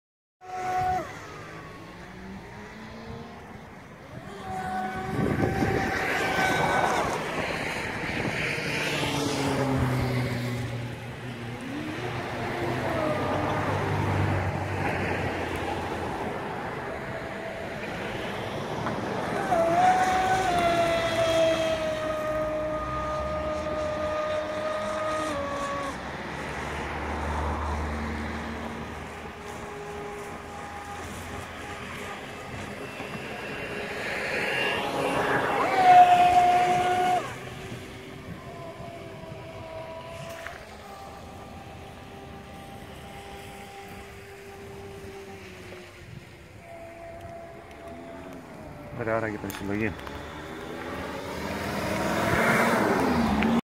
Τηλεκατευθυνόμενο ταχύπλοο μικρό σκάφος
Τηλεκατευθυνόμενο ταχύπλοο μικρό σκάφος στον Όρμο Καθολικού